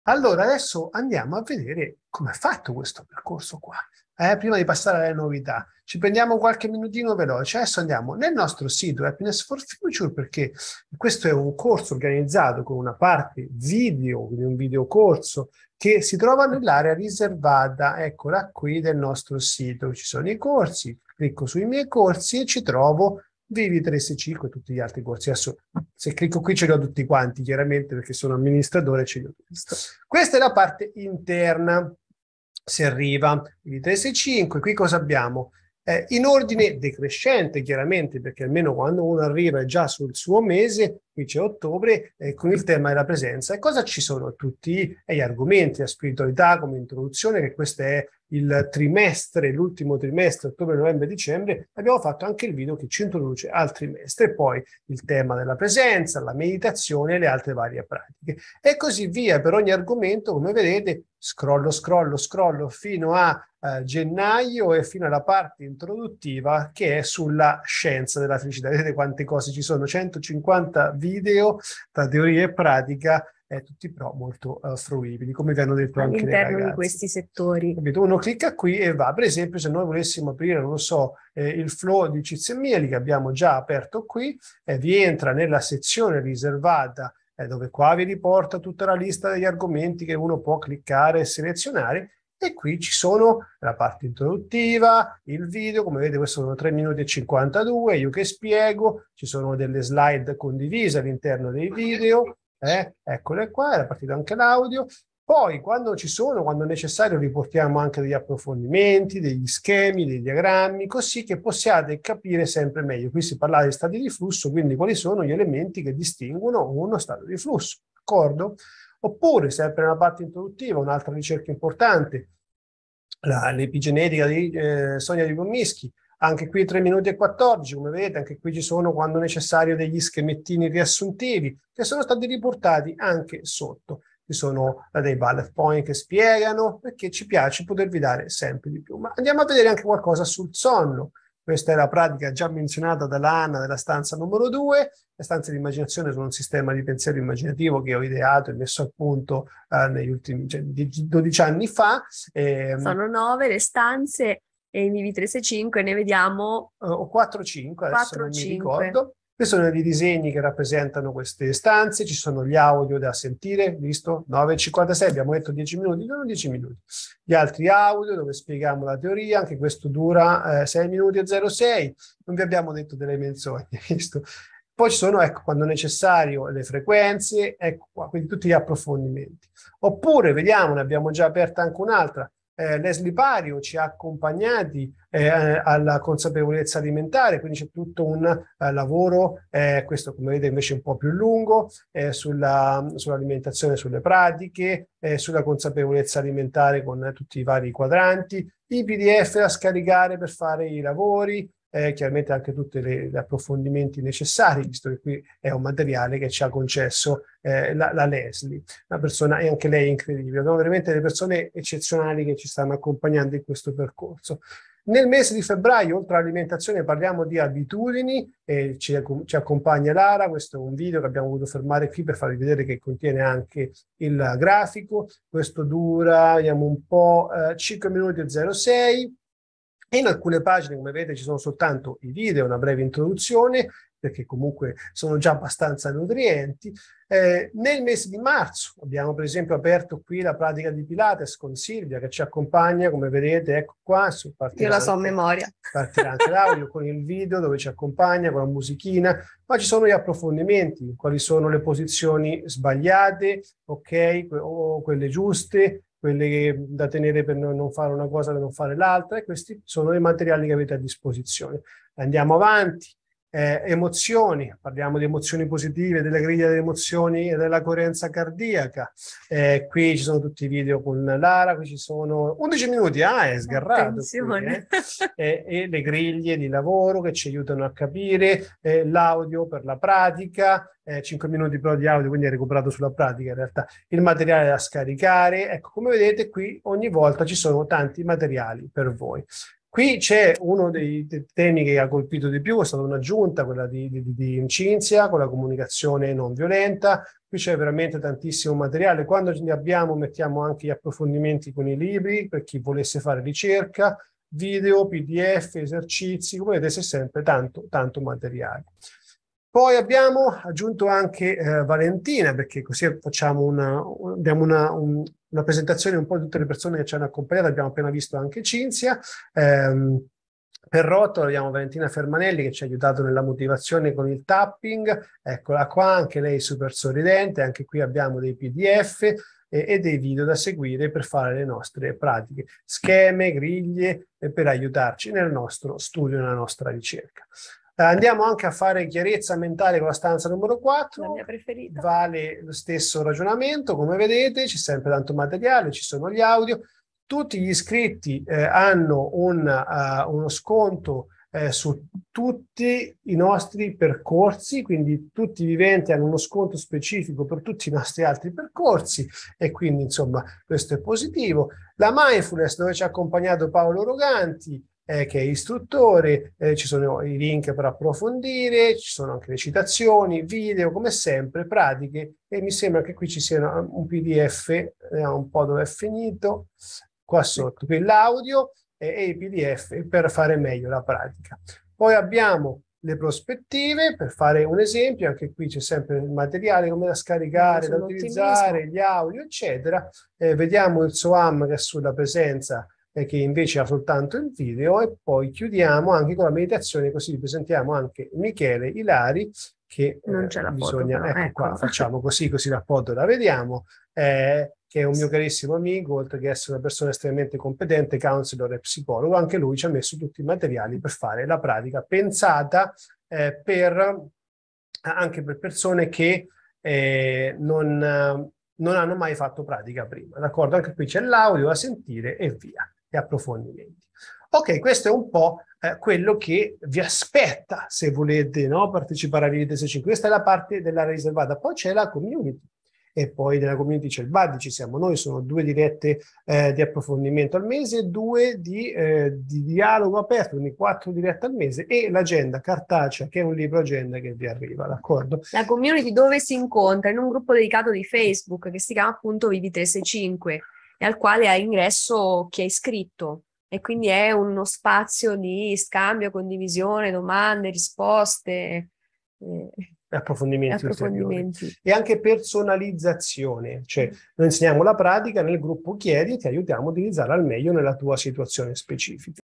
Vivi365 - scopri la piattaforma
Vivi365 - Costruisci la tua felicità. La palestra della Specie Felice:
*1 anno insieme per costruire la tua felicità
*1 piattaforma con oltre 150 video di teorie e oltre 50 pratiche in audio e/o video da far diventare abitudini
*12 mesi, 12 temi su 4 livelli - fisico, emozionale, mentale e spirituale
*1 community - i Viventi -  strepitosa, con persone che hanno a cuore il proprio benessere e il proprio miglioramento
*1 libro-agenda con su cui monitorare i tuoi progressi
*4 dirette al mese - 2 serali, un caffè e un aperitivo
*2 raduni dal vivo
*2 trainer dedicati - Lara Lucaccioni e Matteo Ficara, founder di Vivi365 - e almeno 7 trainer ad approfondire dei temi specifici, come alimentazione, fitness, comunicazione nonviolenta, EFT, mindfulness, meditazione, ascolto attivo.
*la possibilità di un buddy, un alleato, un pari, al mese, con cui condividere e sostenersi a vicenda nel percorso
E' un percorso meraviglioso e ti invitiamo a far parte del movimento della Specie Felice, di cui Vivi365 è la palestra